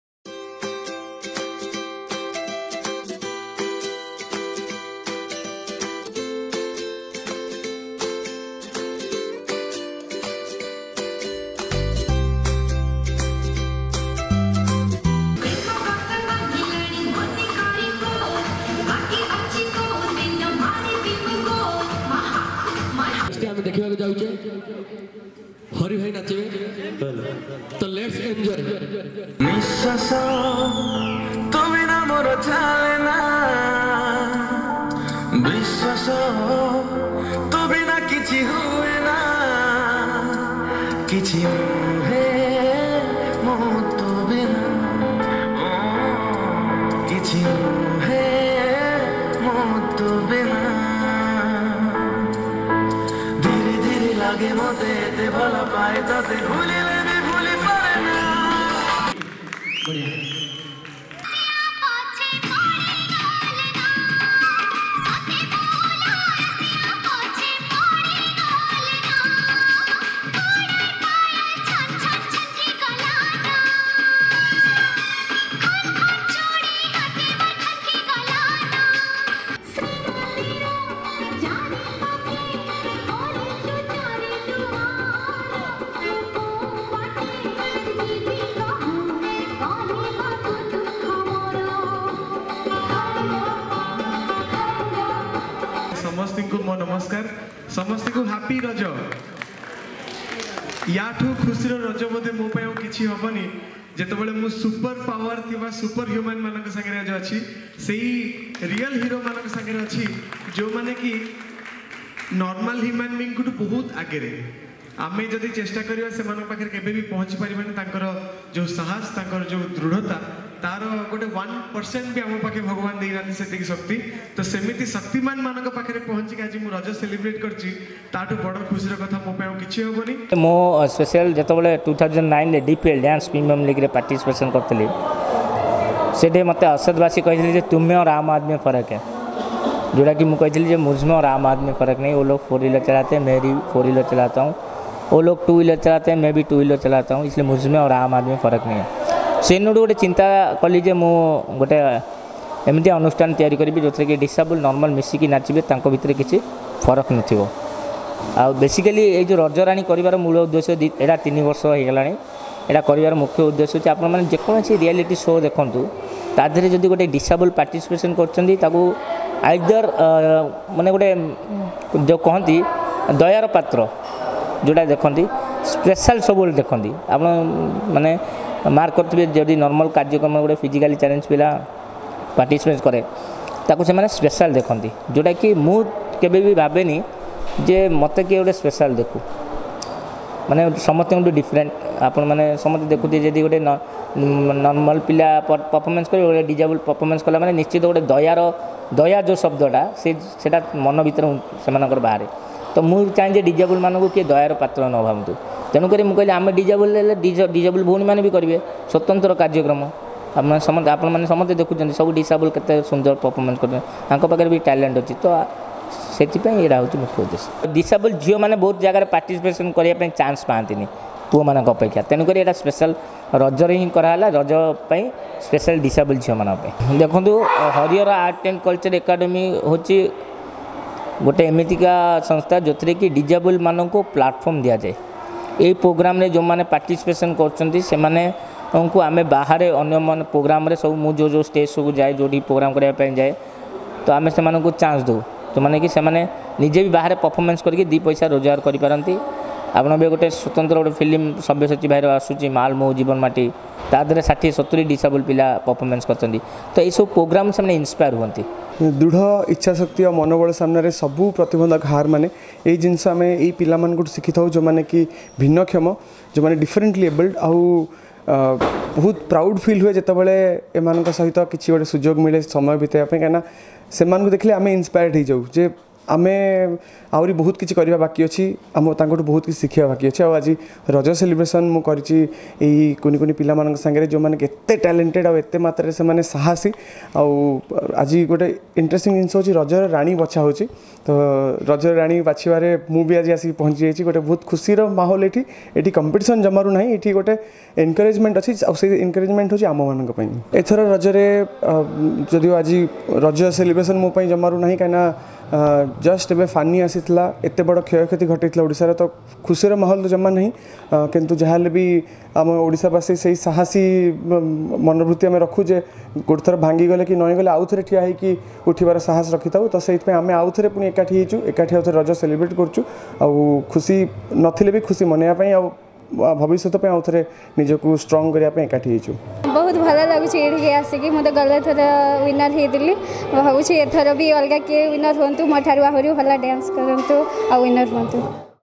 RAJARANI in Pahili Raja | Sabyasachi Mishra | Harihar Deo
The festival of Raja brings with it innumerable celebrations across the twin cities with a wide array of programs for young women, including the ones who prove their mettle despite the society not being really friendly or hospitable to them. Harihar Art and Culture Academy arranged a special program ‘Rajarani’ for the exceptionally-abled at Jayadev Bhavan on June 14, Pahili Raja.